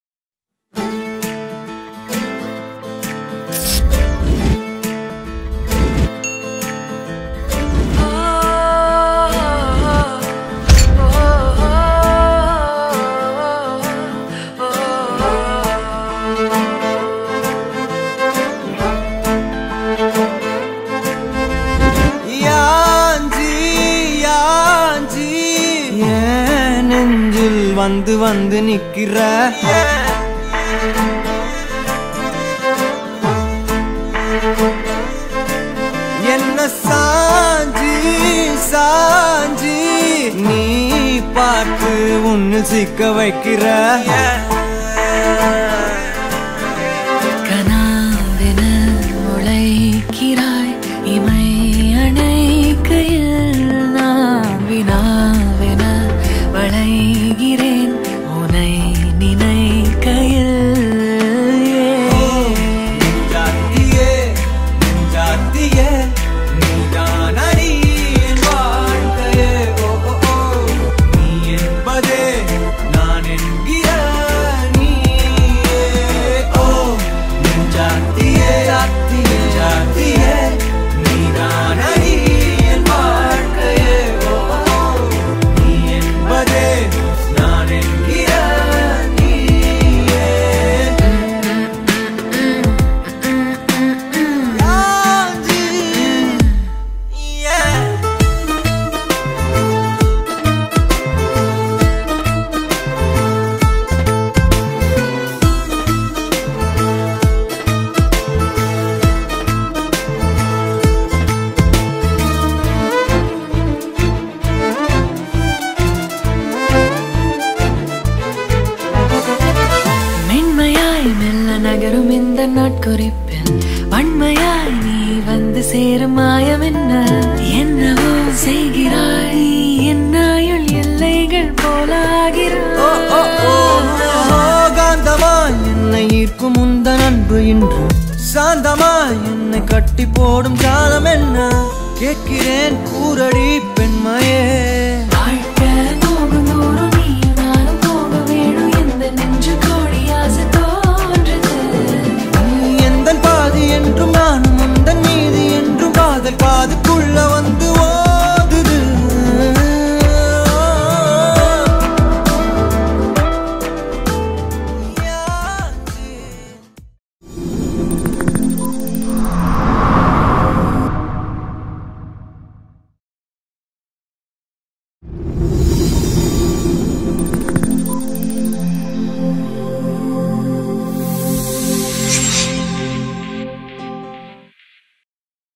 Arjun & Kavya Wedding Highlights - Bangalore | VSG Fotos - Pondicherry
Wedding Highlights of Arjun & Kavya by VSG Fotos, Pondicherry. One of the adorable couples. Watch it and enjoy!